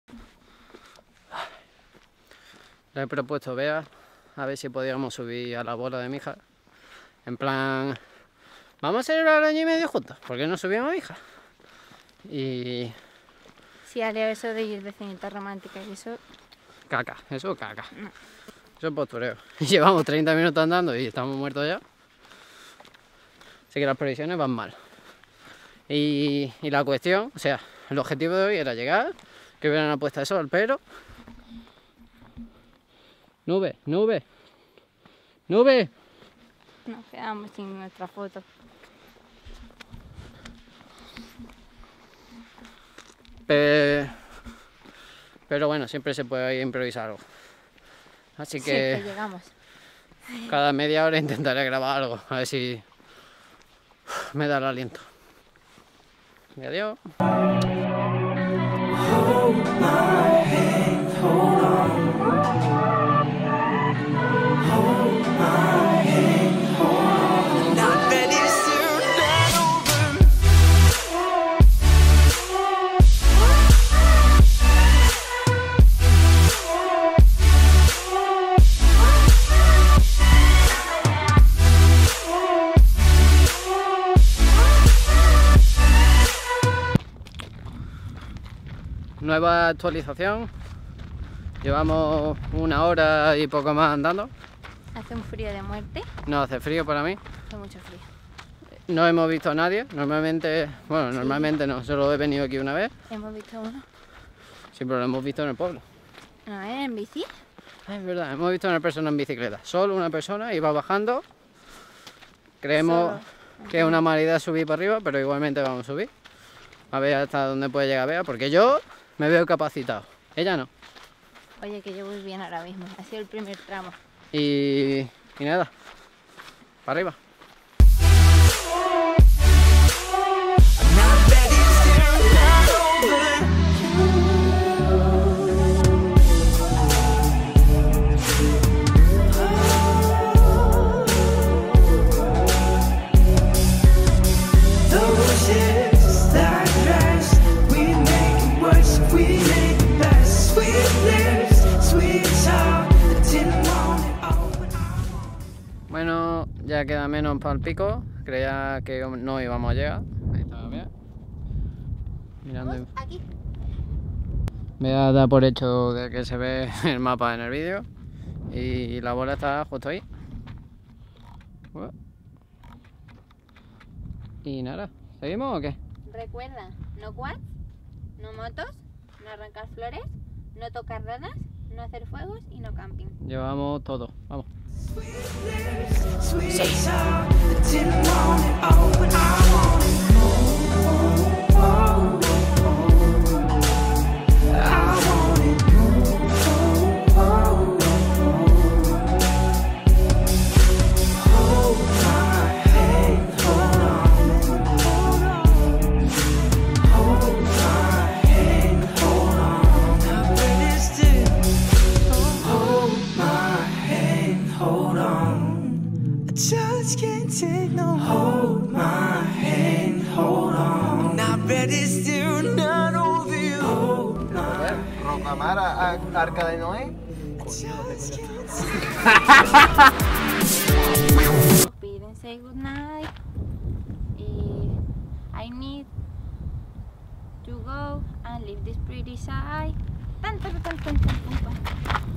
VLOG: LA BOLA ROMÁNTICA
¡Mi canal de YouTube necesita más actividad! Intentaré subir contenido con mayor frecuencia tras los exámenes.

El 11 de Junio de 2015, hacía con Bea 18 meses y como no soy muy fan de las típicas cenas románticas, porque eso lo puedes hacer cualquier otro día, le propuse a Bea que subiésemos al pico de Mijas llamado La Bola para celebrarlo.

PD: Pensaba grabar el día entero porque renové el DNI y tras llegar al pico se nos anocheció muy pronto, nos perdimos y bajamos pegados a una linterna durante 3 horas sin ven nada.

Cámara usada: Canon EOS 6D
Objetivos utilizados: Canon EF 17-40mm f/4, Canon EF 50mm 1.8
Otros: JOBY Gorilapod SLR ZOOM